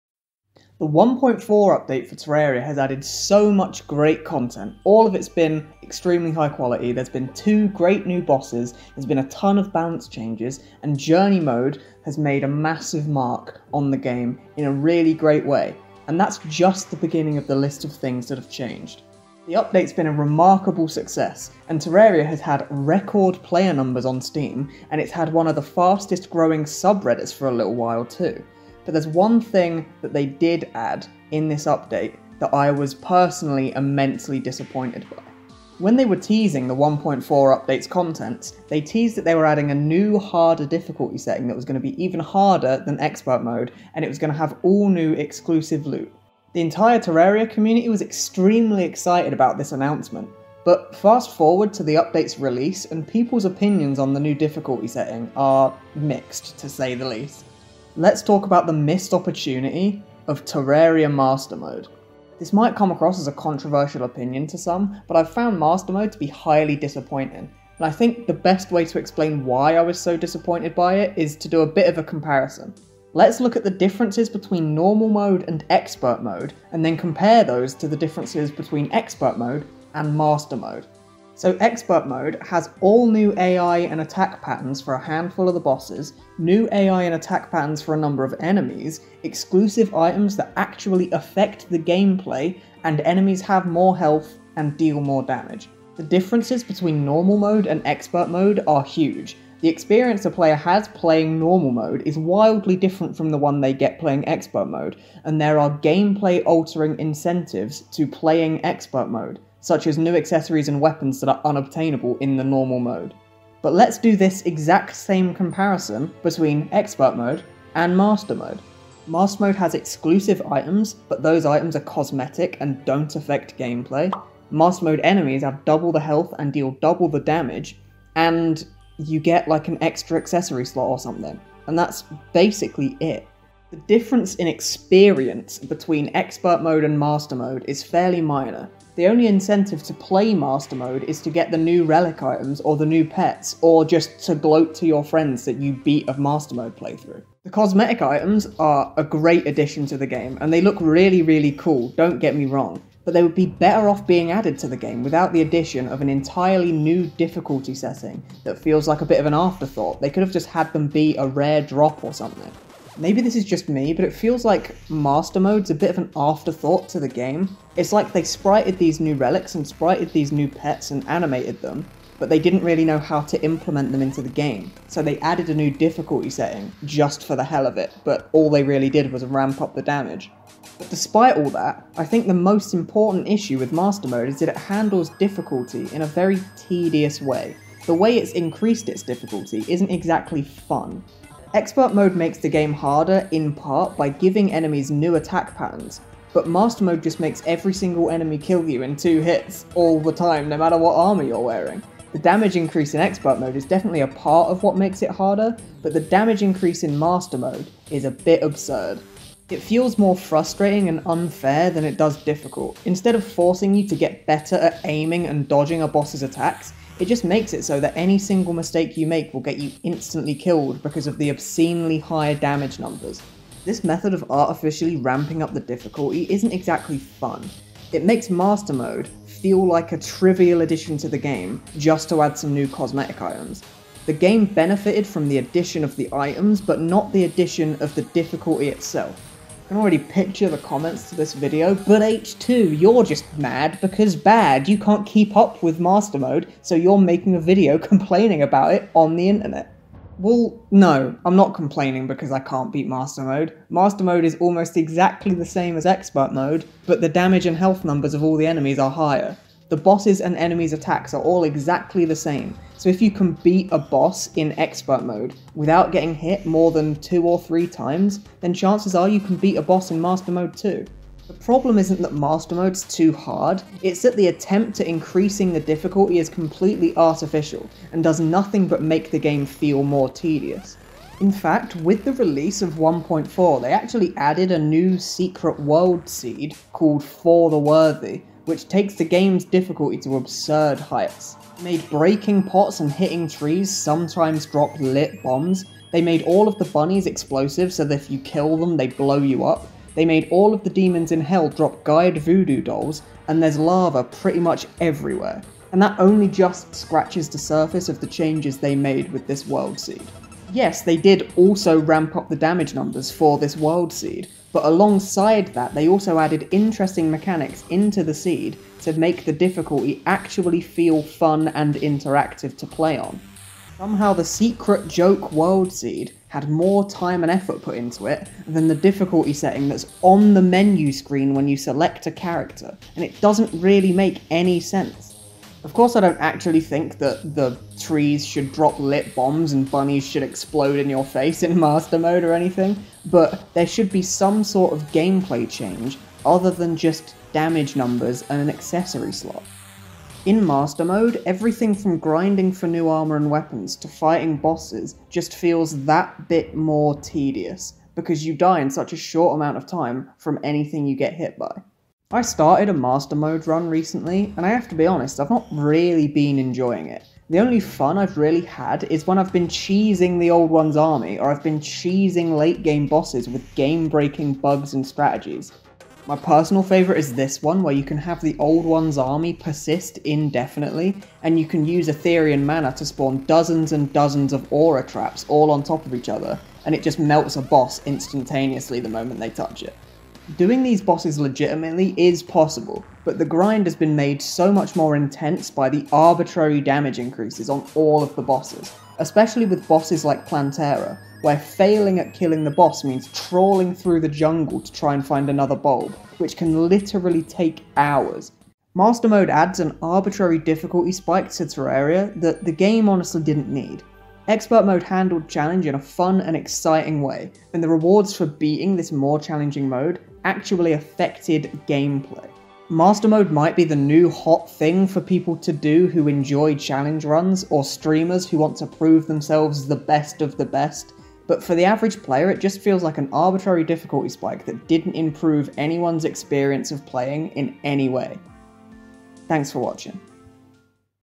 The Disappointment that is Terraria Mastermode
Anyone can make a game that's hard, you just make everything do lots of damage, what's actually hard is to make it difficult without being mind numbing and tedious